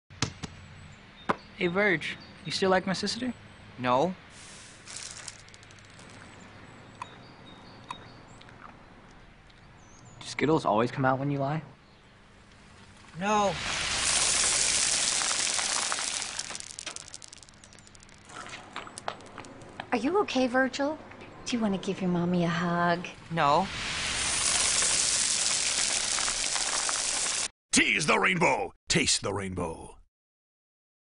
Funny Skittles Commercial Liar, Liar
skittles comes out of this kids ears when he tells a lie... sweet how many times a day would skittle come out of your ear?